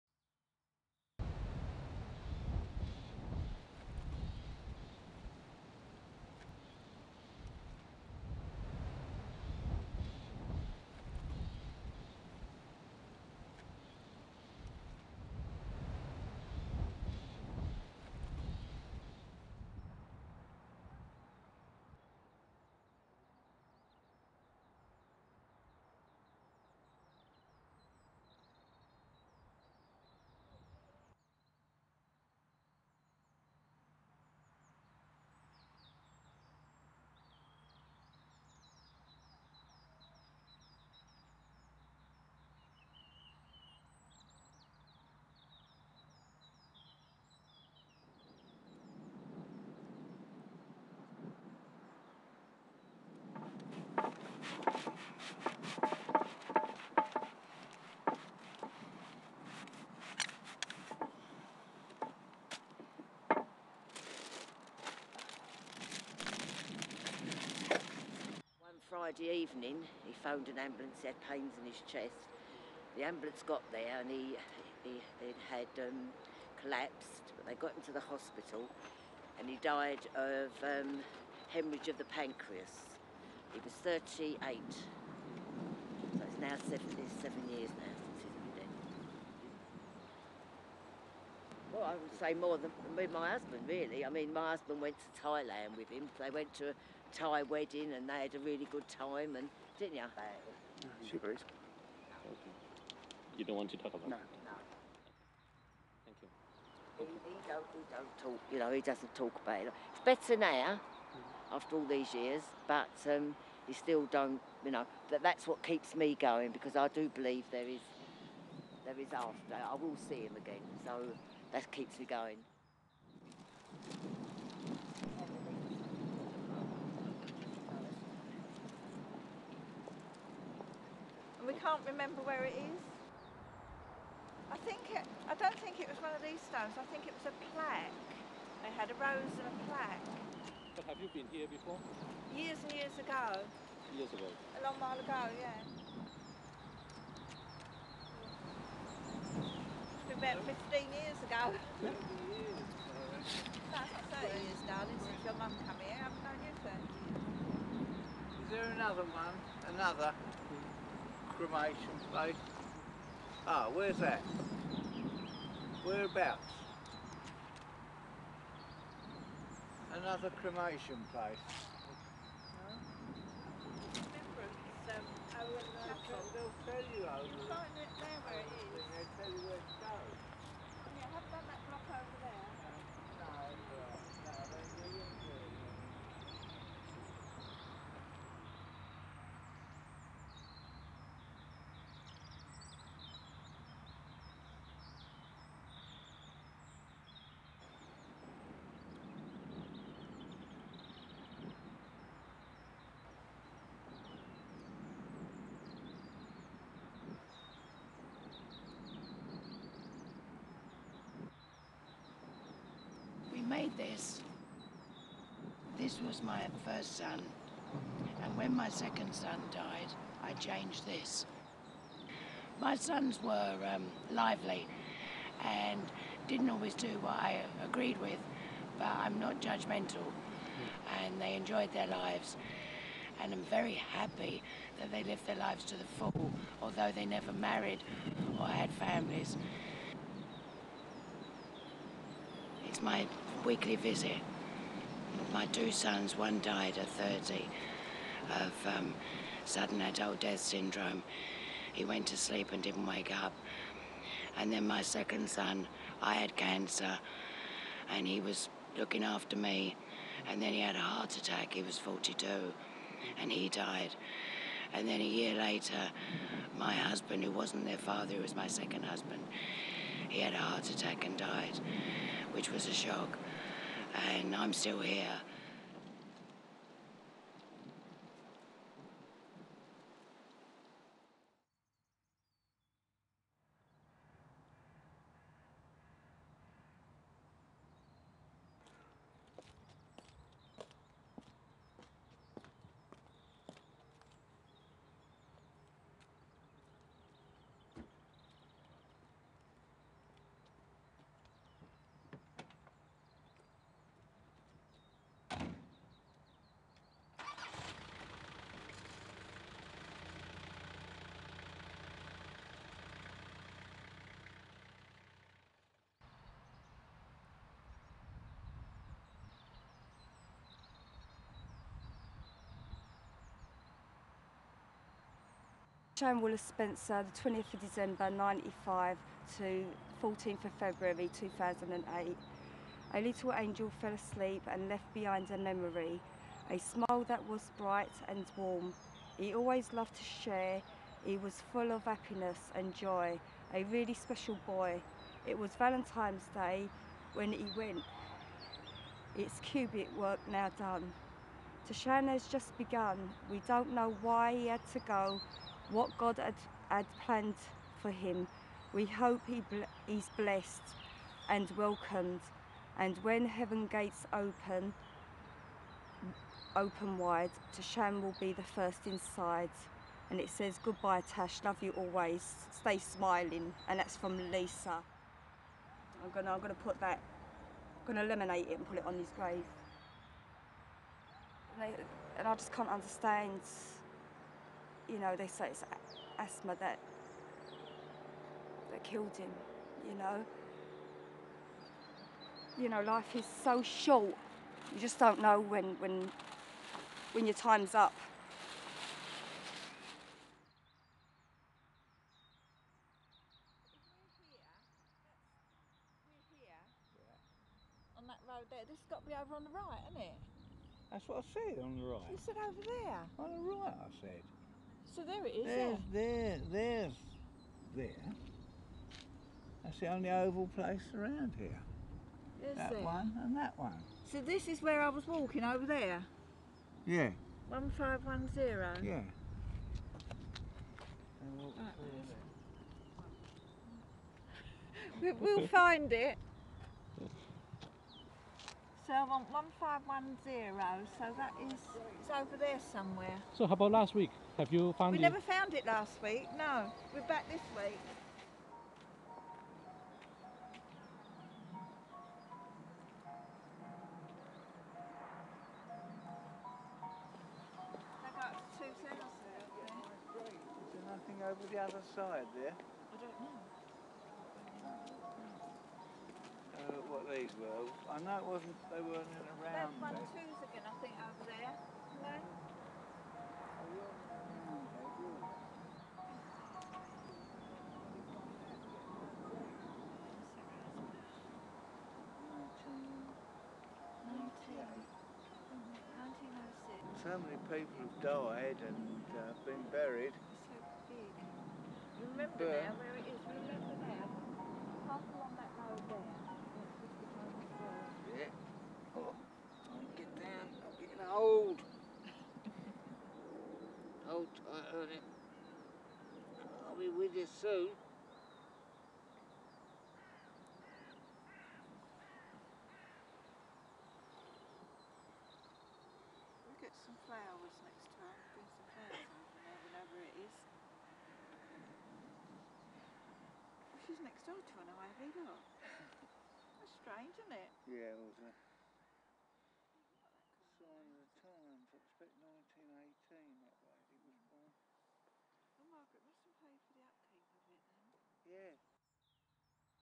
The Memory's Palace documentary(9 mins)
That's the greatest cemetery in London, with 150 thousands graves which means 150 thousands stories was inside there. I went to the cemetery for 12 days, stayed there and talked with the people. The stoires were not ultimately sad, but also with happiness and humour.